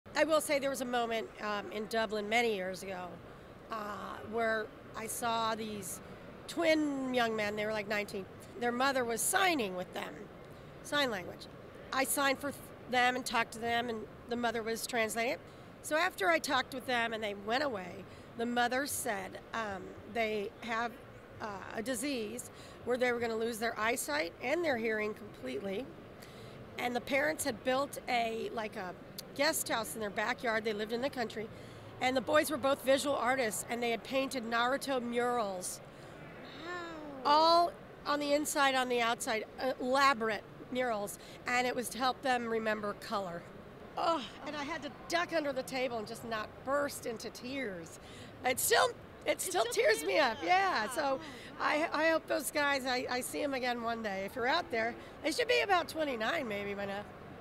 Voice of naruto Maile Flanagan shares BEST FAN MOMENT
Maile Flanagan, (Naruto English dub voice actor, Lab Rats) joins Popverse's Veronica Valencia to discuss the BEST characters, moments, and even apparel from the popular shounen anime. Naruto vs. Sasuke, what's the best fight? Who's the best Naruto villain? Best jutsu? Maile has thoughts, and even shares heartwarming fan stories, and helpful advice to anyone looking to meet her at a future convention!